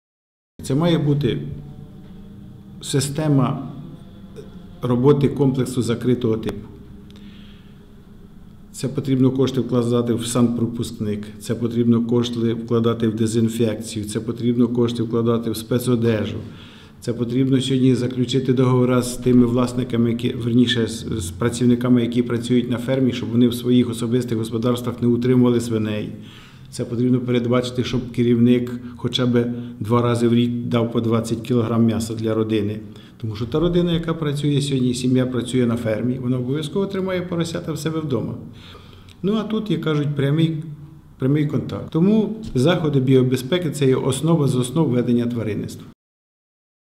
Нехтування санітарними нормами – основна причина спалаху африканки на Хмельниччині 2
Нехтування санітарними нормами – основна причина спалаху африканської чуми у Теофіпольському районі. Про це в ексклюзивному інтерв’ю нашому каналу повідомив начальник головного управління держпромспоживслужби області Василь Грушко.